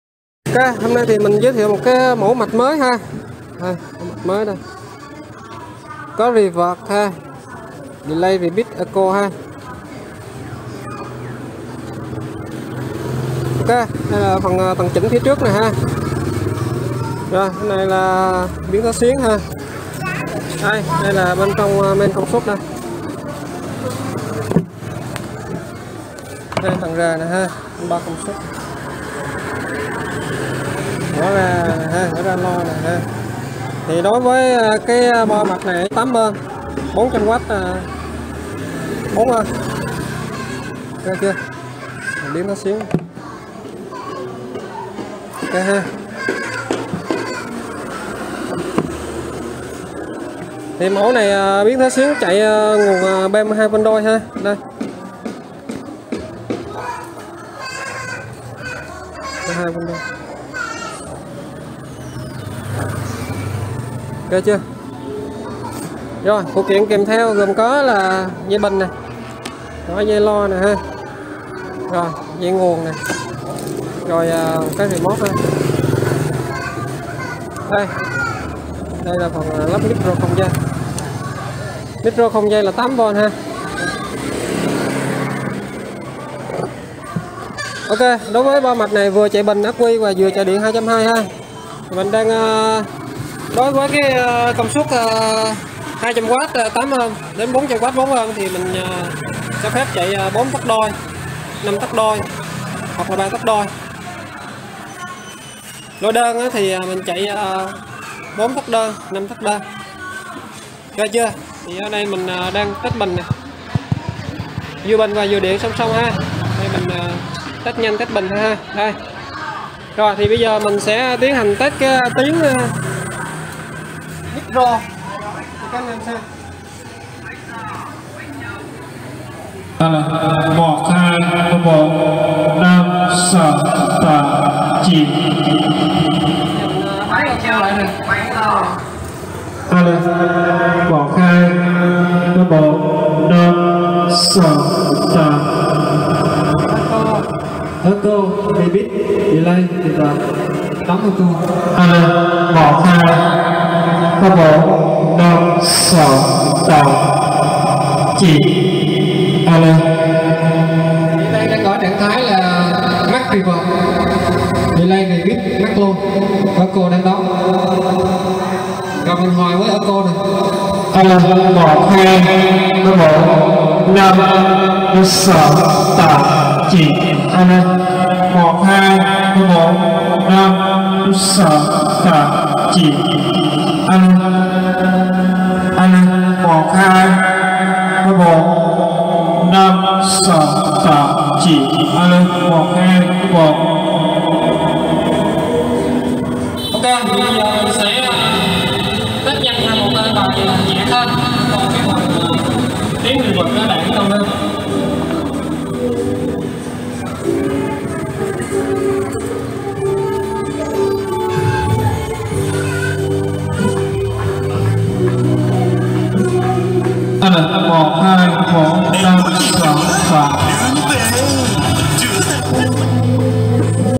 Mạch loa kéo Reverb DSP chạy FET công suất 200W 8ôm – 400W 4ôm, loa kéo 5 – 4 – 3 tấc đôi hoặc đơn
Mạch loa kéo Reverb DSP chạy FET công suất 200W 8 ôm – 400W 4 ôm, loa kéo ĐÔI 5 – 4 – 3 tấc hoặc loa kéo ĐƠN 5 – 4 tấc
Mạch loa kéo mẫu mới 2022 DSP Reverb – Echo chạy FET công suất lớn cho loa kéo ĐÔI hoặc Đơn.
- Kích thước tấm sắt: 19 x 38cm. Chiều sâu mạch 10cm
- Có tổng cộng 11 Volume chỉnh:
    + Karaoke: M.Vol – M.Bass – M.Treble – Reverb – Delay – Repeat – Echo
    + Nhạc: Volume – Bass – Treble
    + Guitar: G.Vol
- Mạch chạy FET có công suất 200W 8 ôm – 400W 4 ôm. Dùng cho loa kéo 5- 4- 3 tấc ĐÔI hoặc 5 – 4 tấc ĐƠN. Có 2 cách lắp loa: LOA ĐÔI – 2 Bass 5 tấc (4 tấc hoặc 3 tấc) + 2 Trung + 2 Trung/ LOA ĐƠN: 1 Bass 5 tấc (hoặc 4 tấc) + 1 Trung + 1 Treble
- Mạch sử dụng Bluetooth đời mới – có chức năng TWS kết nối 2 mạch với nhau. Màn hình hiển thị Có đèn báo dung lượng Bình. Đọc thẻ nhớ và USB, có FM.
- Mạch có biến thế xuyến lắp trực tiếp trên Mạch dùng cho chạy điện 220V. Mạch dùng bình ắc qui 12V có sạc tự động ngắt (cắm điện 220V để sạc). Có 2 đèn báo CHA và LOW
- Bus 4 chân lắp Micro không dây: OK KC +8V GND. 2 cổng vào Micro có dây và 2 cổng Guitar
- Có ngõ vào tín hiệu INPUT AUDIO (kết nối TI VI) và ngõ ra tín hiệu OUTPUT AUDIO
- Bảng mạch bao gồm 3 thành phần chính: Tầng chỉnh + phần công suất (lắp trên khung nhôm để main công suất và tản nhiệt) + biến áp xuyến
Trọn sản phẩm bao gồm: 1 MẠCH LOA KÉO Reverb + 1 Remote + 1 dây nguồn 220 + 1 dây bình ắc qui